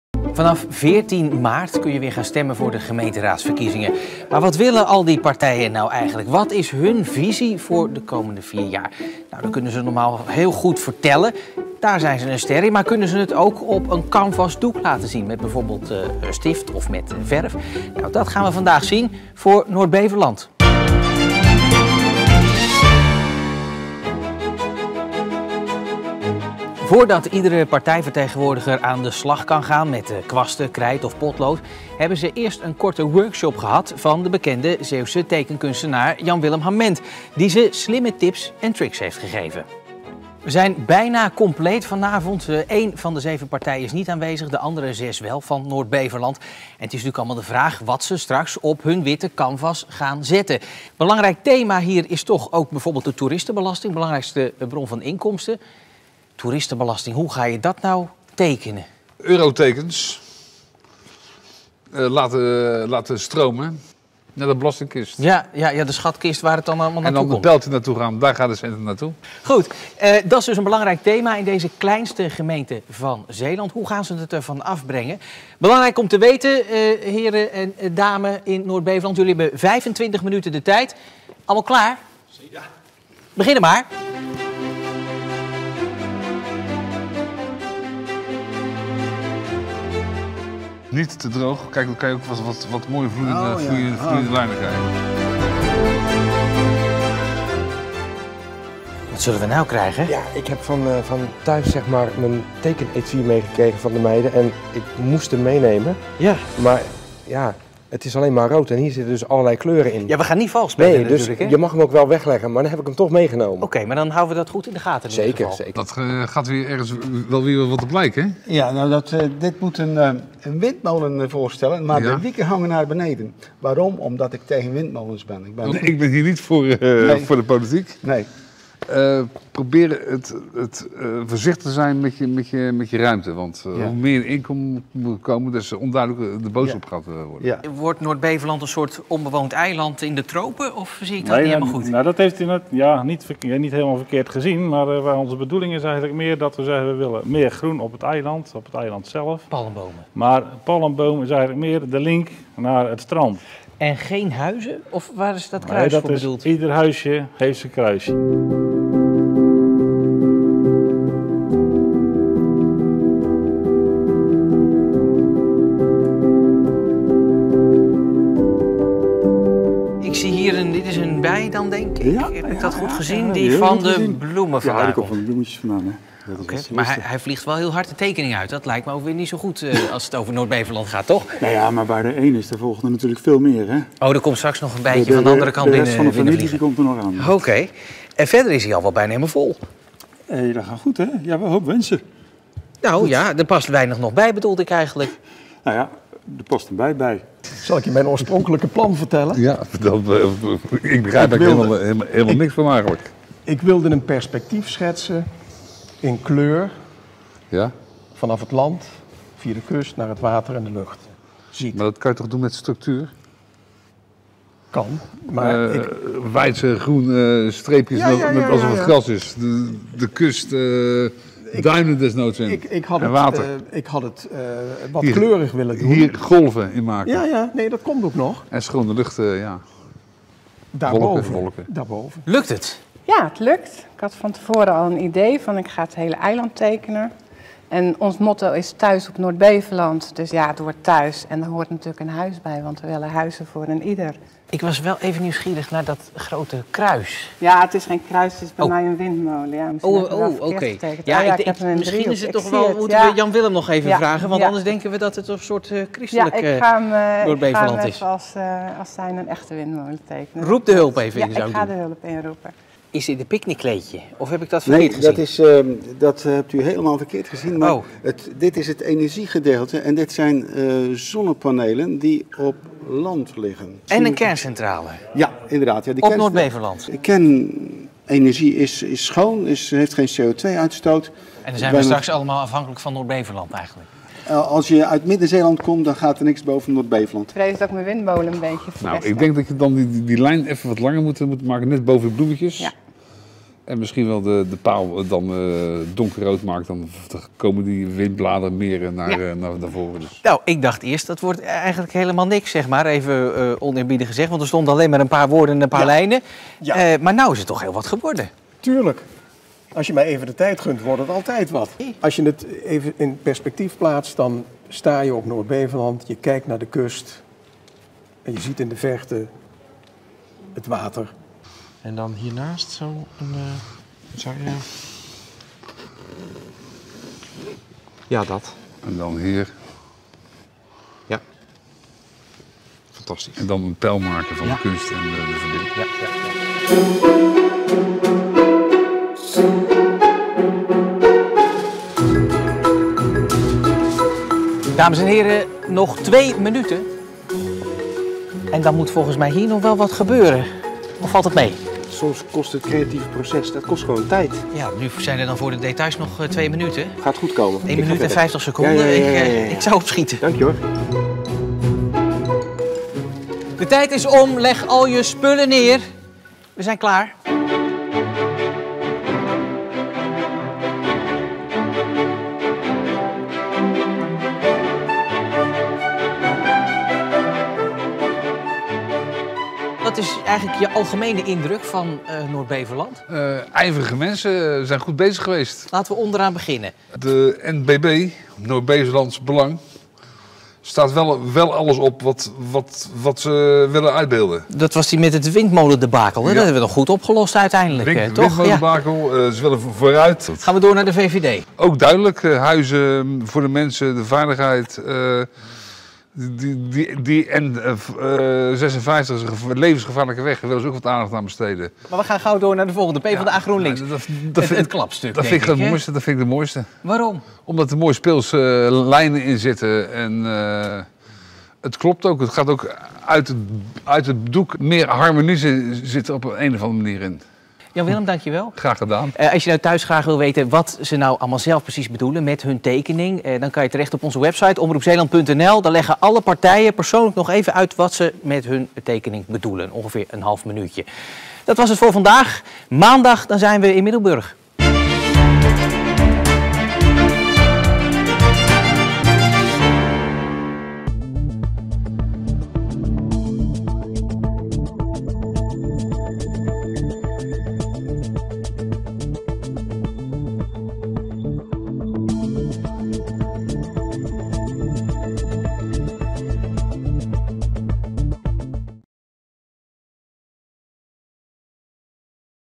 Trots op Zeeland, Cees Priem
Portretten van trotse Zeeuwen. Waarom kiezen zij voor Zeeland? En wat maakt het zo leuk om hier te wonen?